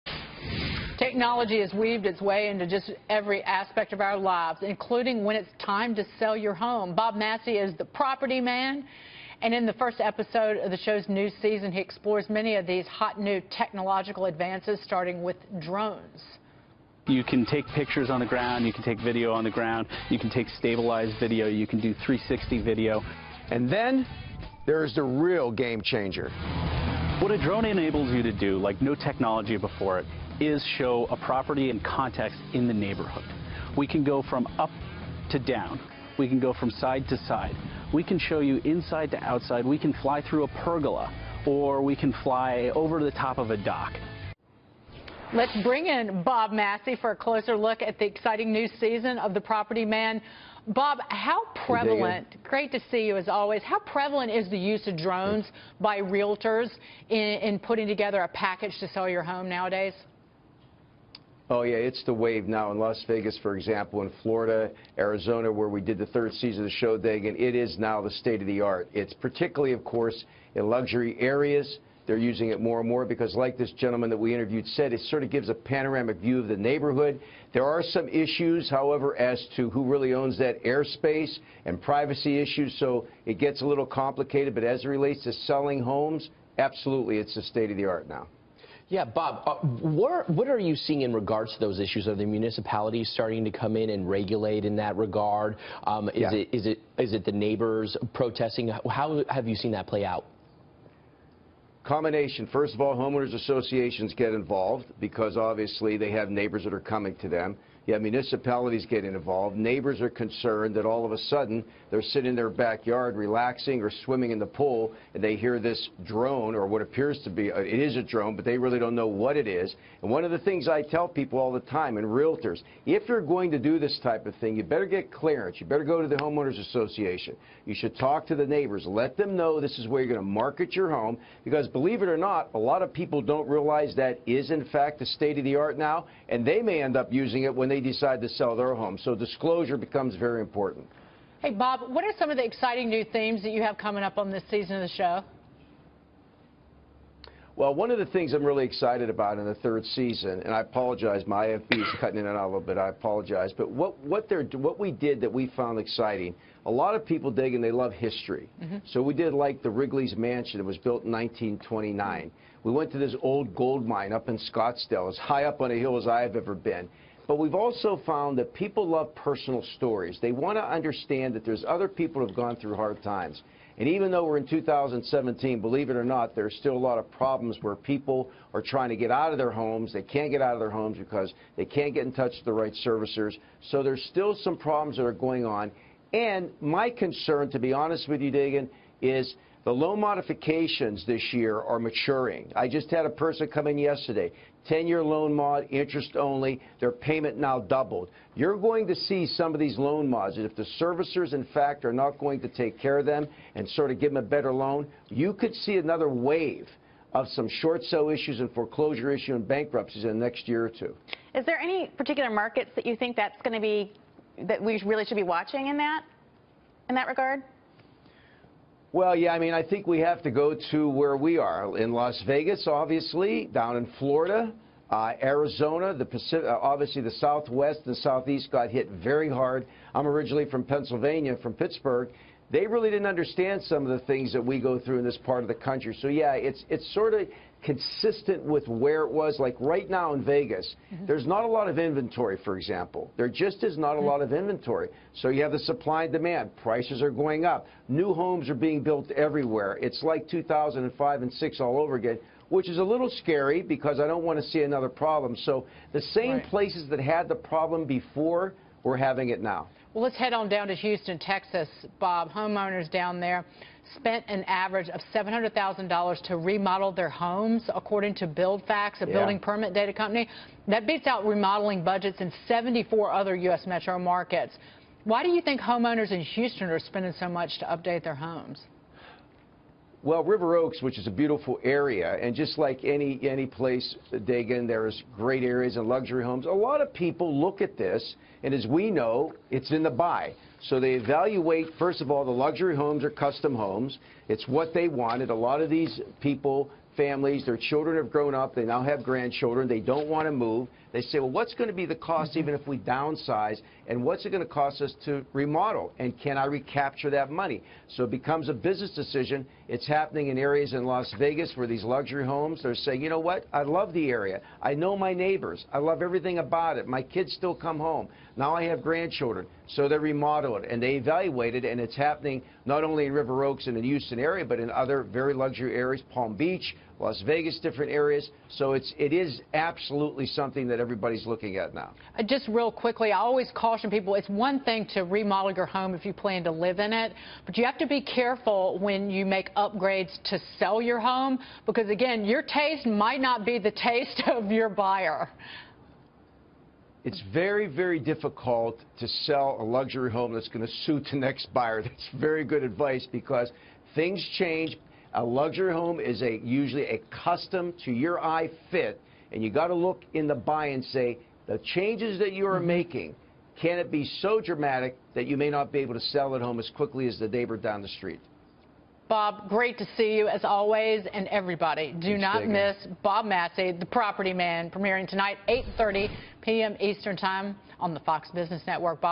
Using drones to market your home
‘The Property Man’ Bob Massi on the growing use of technology such as drones to sell real estate.